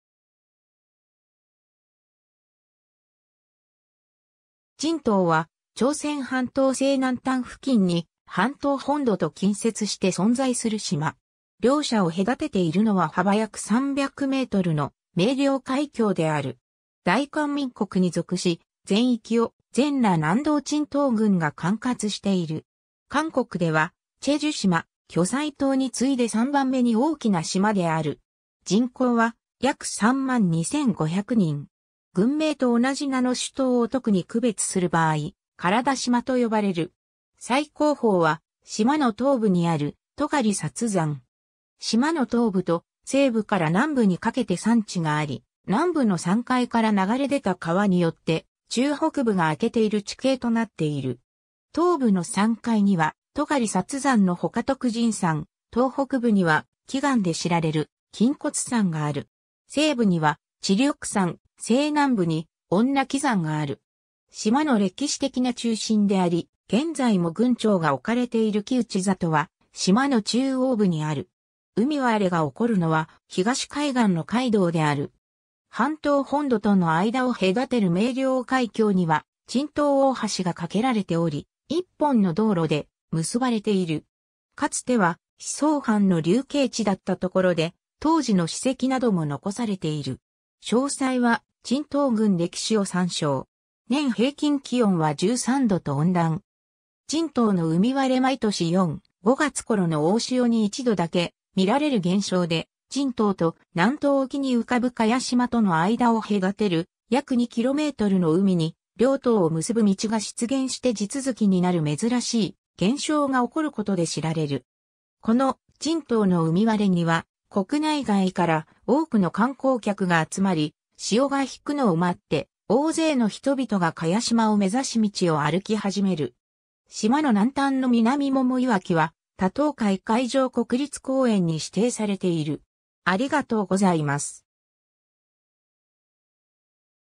珍島
珍島（チンド、ちんとう、朝: 진도）は朝鮮半島西南端付近に、半島本土と近接して存在する島。
両者を隔てているのは幅約300mの鳴梁海峡である。
大韓民国に属し、全域を全羅南道珍島郡が管轄している。
韓国では済州島・巨済島に次いで3番目に大きな島である。
人口は約3万2500人。
郡名と同じ名の主島を特に区別する場合、体島（チェド/體島/체도、「本島」の意）と呼ばれる。
 最高峰は島の東部にある尖察山（チョムチャルサン/尖察山/첨찰산、485m）。
 島の東部と、西部から南部にかけて山地があり、南部の山塊から流れ出た川によって中北部が開けている地形となっている。
東部の山塊には尖察山のほか徳神山（トクシンサン/德神山/덕신산、385m）、東北部には奇岩で知られる金骨山（クムゴルサン/金骨山/금골산、193m）がある。
西部には智力山（チリョクサン/智力山/지력산、325m）、西南部に女貴山（ヨグィサン/女貴山/여귀산、457m）がある。
 島の歴史的な中心であり現在も郡庁が置かれている城内里は島の中央部にある。
海割れが起こるのは東海岸の回洞である。
 半島本土との間を隔てる鳴梁海峡には珍島大橋が架けられており、1本の道路で結ばれている。
 かつては思想犯の流刑地だったところで、当時の史跡なども残されている。
 詳細は珍島郡#歴史を参照。
 年平均気温は13度と温暖。
 珍島の海割れ 毎年4、5月頃の大潮に1度だけ見られる現象で、珍島と南東沖に浮かぶ茅島との間を隔てる約2 kmの海に、両島を結ぶ道が出現して地続きになる珍しい現象が起こることで知られる。
この珍島の海割れ（別名:神秘の海割れ）には国内外から多くの観光客が集まり、潮が引くのを待って大勢の人々が茅島を目指し道を歩き始める。
 島の南端の南桃石城は多島海海上国立公園に指定されている。